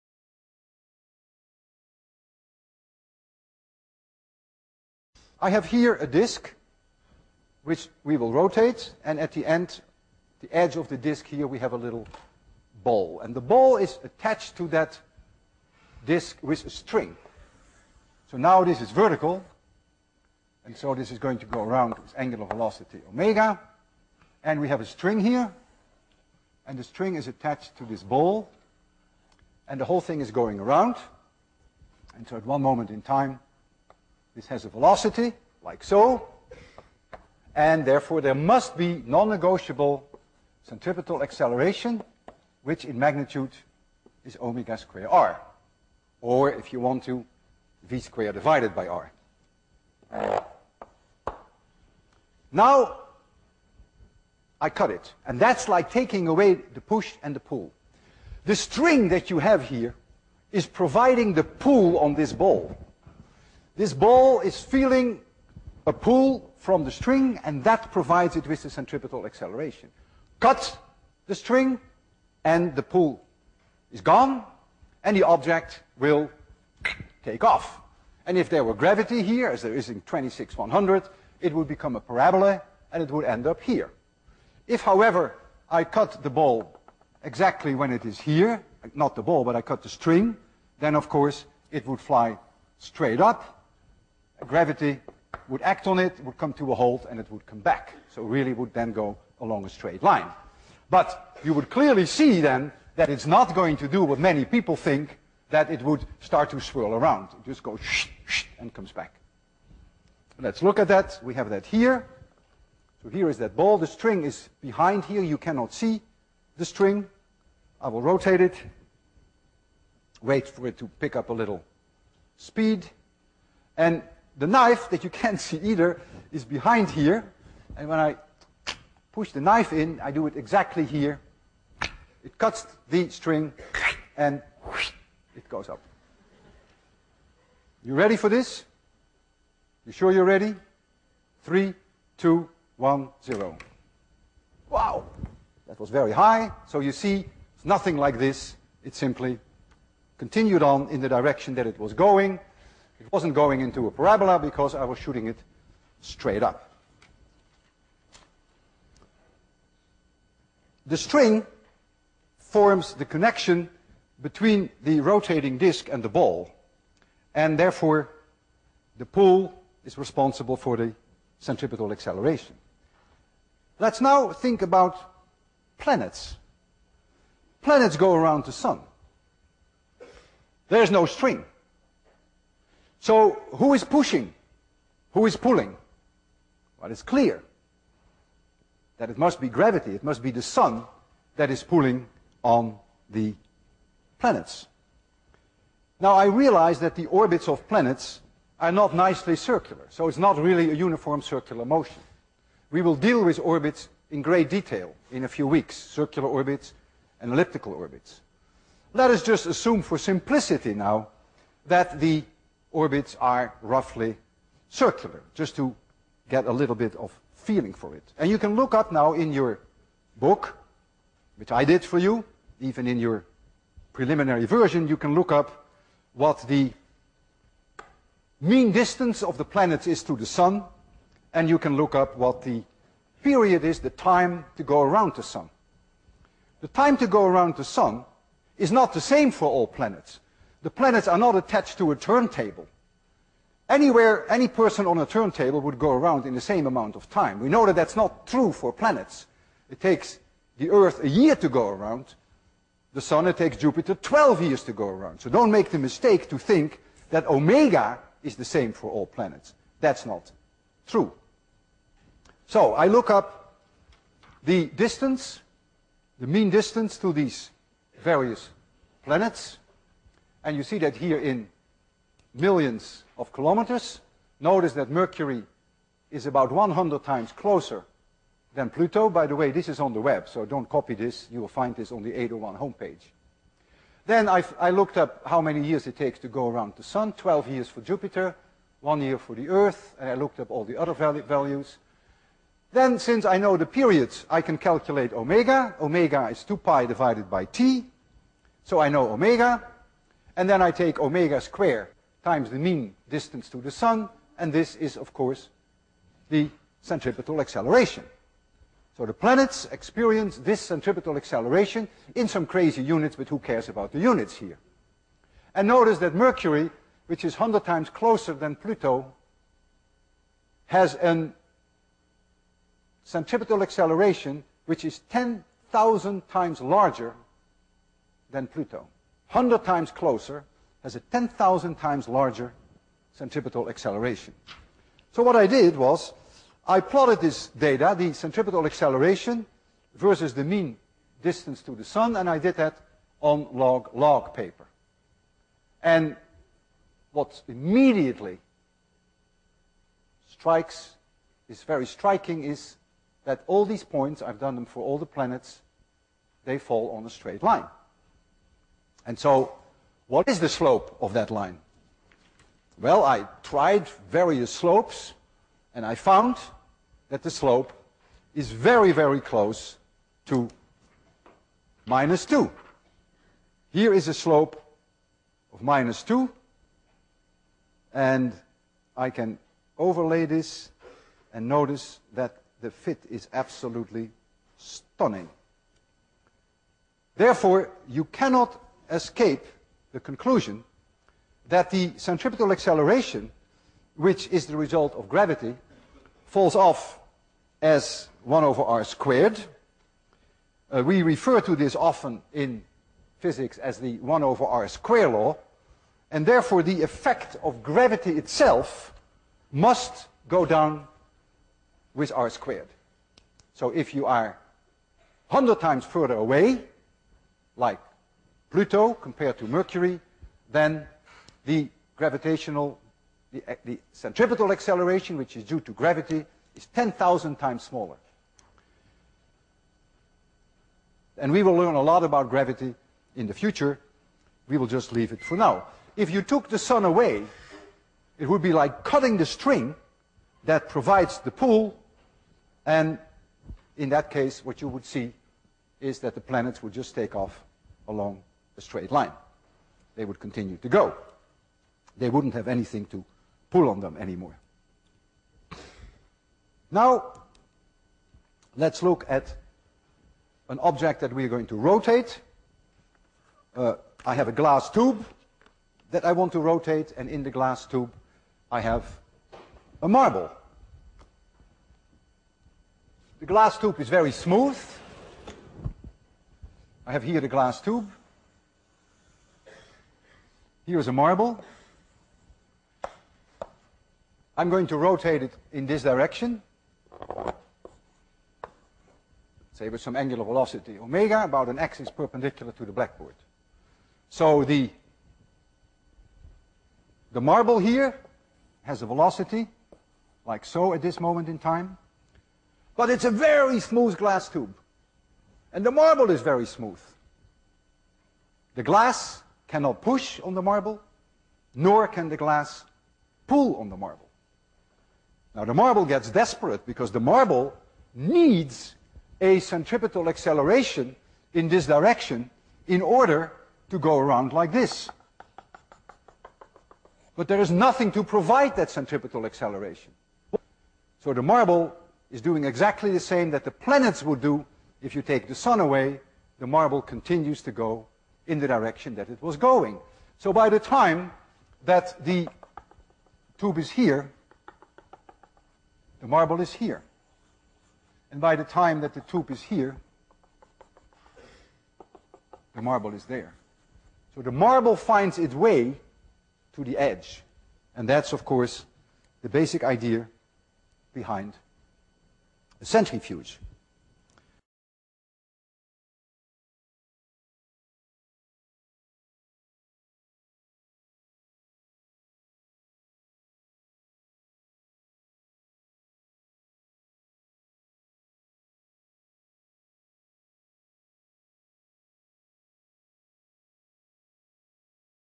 Chương V_2: Chuyển động tròn, chuyển động li tâm........
Chương V, phần 2: Chuyển động tròn, chuyển động li tâm, hệ quy chiếu, trọng lực biểu kiến. dịch và làm phụ đề.